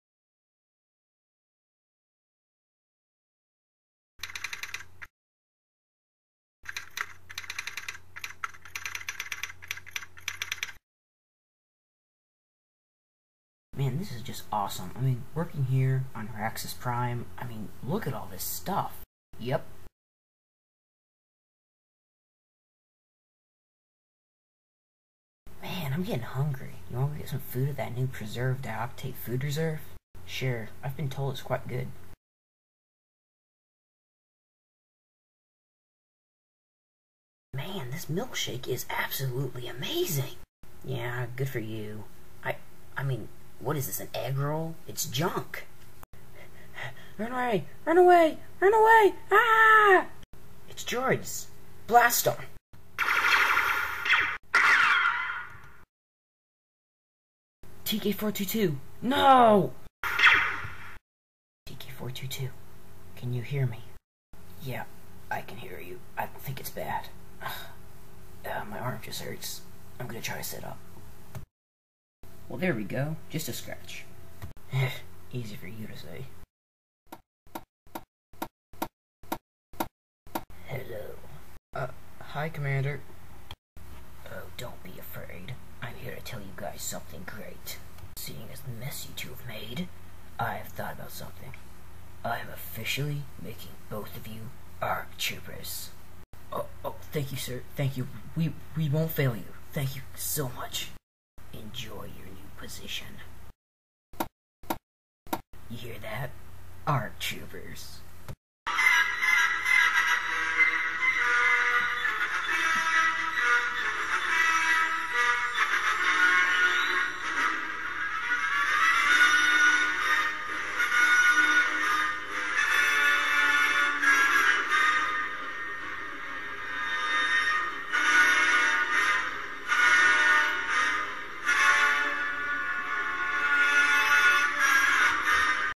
lego star wars TK-421 a real story episode 3, One Trashy Day!
No copyright is intended! All music by John Williams.

TK-421 and TK-422 soon come to find out their dream job is more than they bargained for on Raxus Rrime. When droids attack and food is junk this is one trashy day.